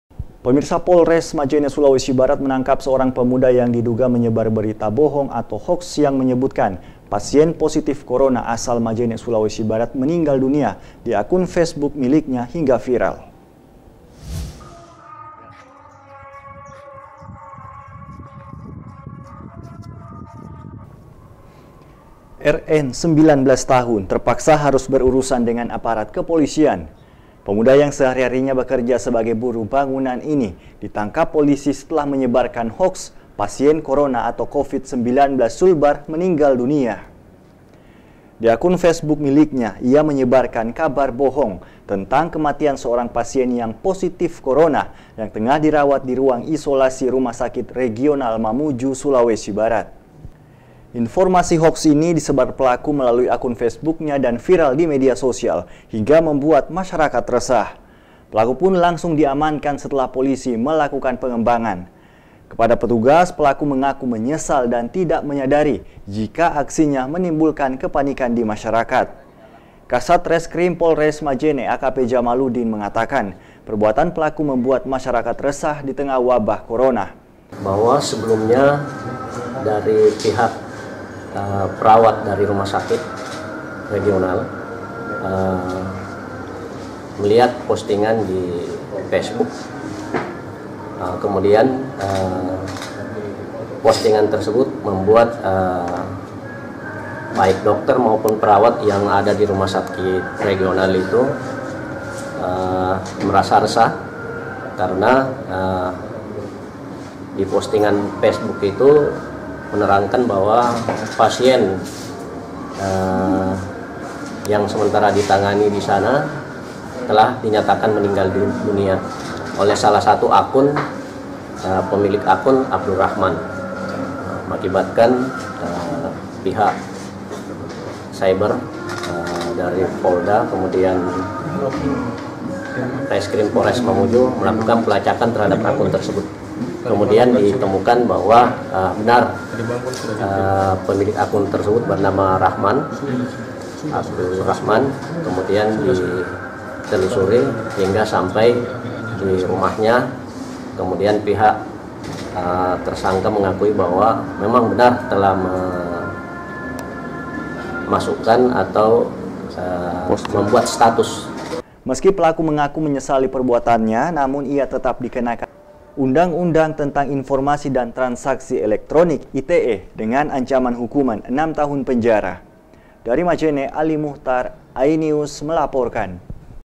Penyebar Hoax Corona Ditangkap | iNews Sulbar | 02-04-2020
Polres majene sulawesi barat menangkap seorang pemuda yang diduga penyebar berita bohong, atau hoax yang menyebutkan, pasien positif corona asal majene, sulawesi barat, meninggal dunia, di akun facebook miliknya, hingga viral.
Kontributor : Alimukhtar